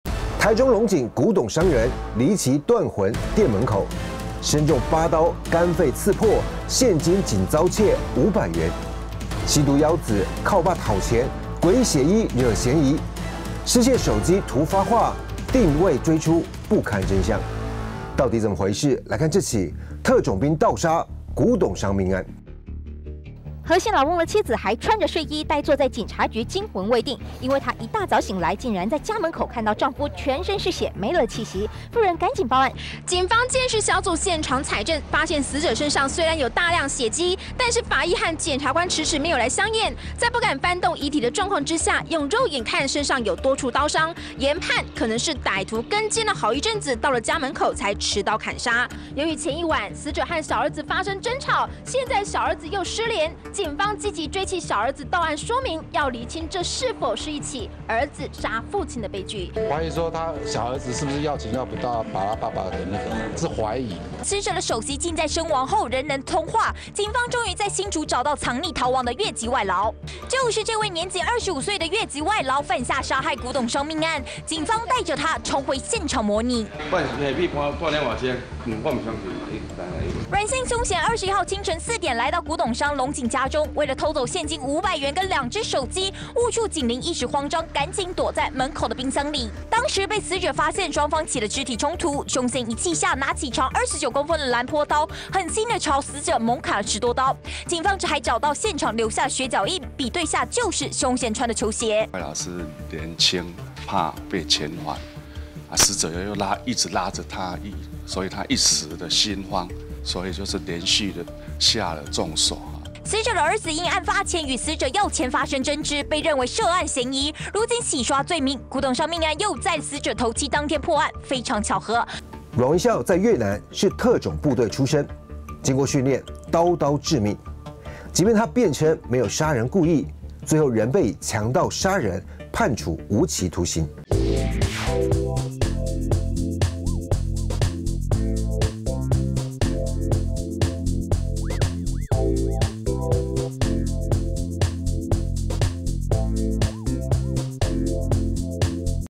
【速讀案336】忤惡!海派骨董商慘死店門口 染毒么兒成最大嫌疑犯
2008年9月21日，台中龍井中港路上(後來改名台灣大道)一間骨董店，凌晨六點一名婦人穿著睡衣，連鞋子也沒穿衝上馬路，攔住一輛計程車借電話要報警，因為她的丈夫身中8刀倒臥血泊，而她最懷疑的兇手竟是染毒的小兒子，到底怎麼回事，08/28 21:00,鎖定《忤惡-老Z調查線》YT頻道，帶您揭密。

●老Z新書《誰養出的魔鬼》各大實體書店及網路通路全面上架